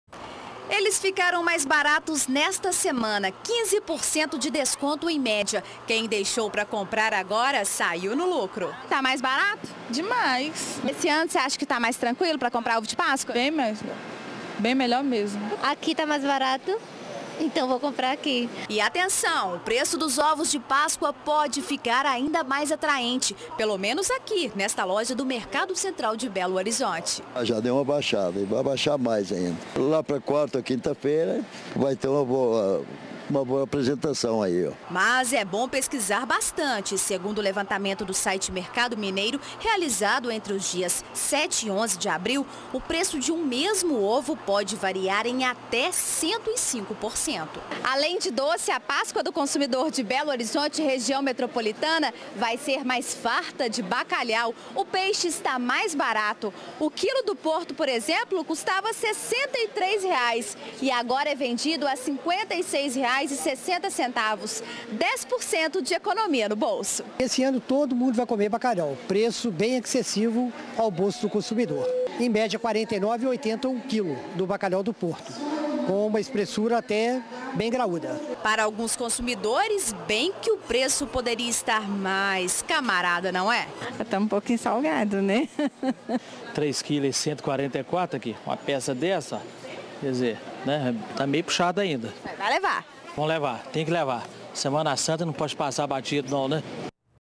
Preços de ovos de páscoa e peixes caem no comércio da capital
Com a chegada da Páscoa, o preço dos ovos de chocolate caiu, em média, 15%. Outros produtos da época, como os peixes, também estão mais baratos.